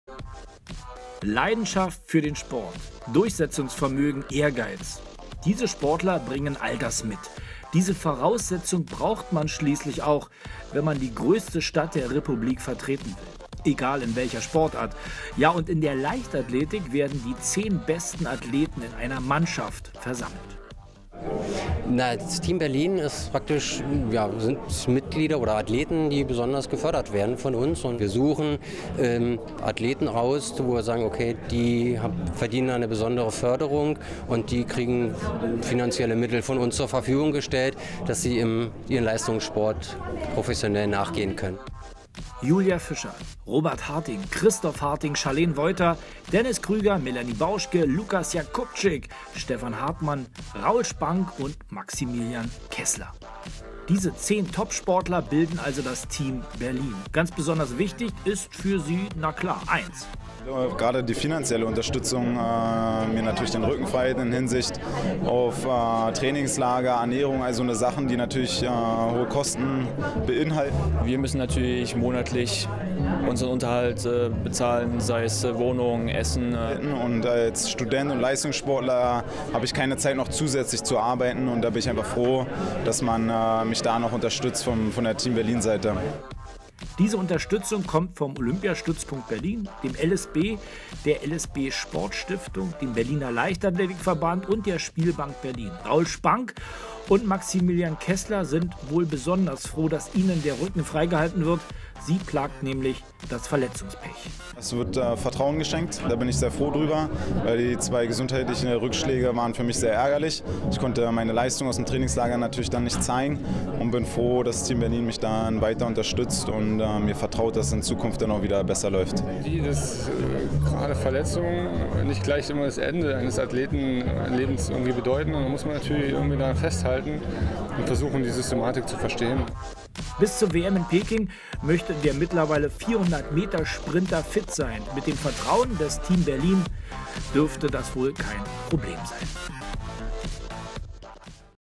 Präsentation des Team Berlin
Leidenschaft für den Sport, Durchsetzungsvermögen, Ehrgeiz. Diese Sportler bringen all das mit! Diese Voraussetzung braucht man schließlich auch, wenn man die größte Stadt der Republik vertreten will-egal in welcher Sportart. Ja und in der Leichtathletik werden die 10 besten Athleten im Team Berlin versammelt.